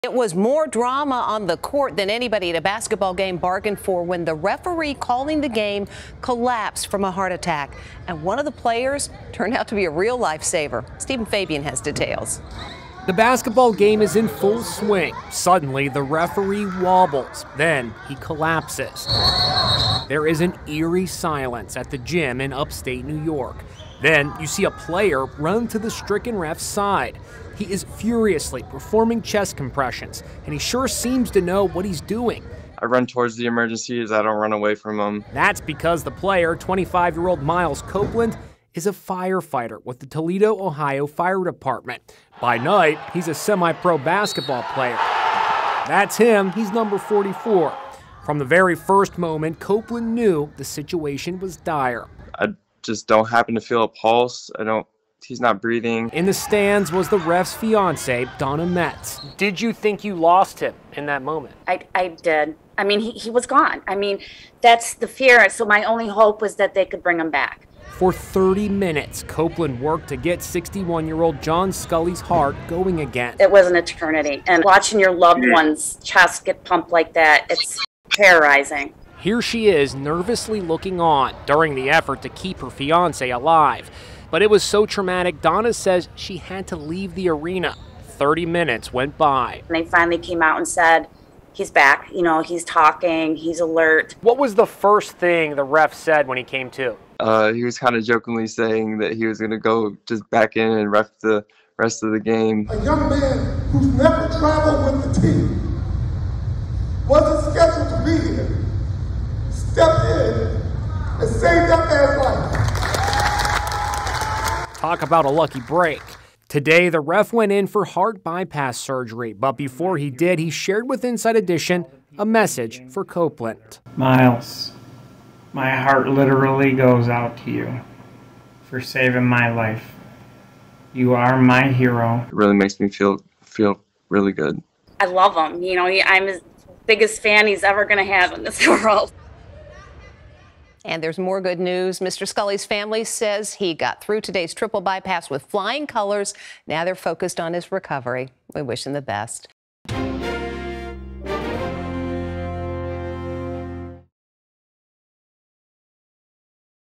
Basketball Player Saves Referee’s Life During a Game
A basketball game in Upstate New York is in full swing when suddenly the referee wobbles and collapses from a heart attack. That's when Toledo Glass City basketball player Myles Copeland jumps into action. As luck would have it, Copeland is not just a basketball player — he's a firefighter at the Toledo, Ohio, fire department. He starts to perform CPR on 61-year-old John Sculli. For 30 minutes, Copeland worked to get Suclli's heart going again, ultimately saving his life.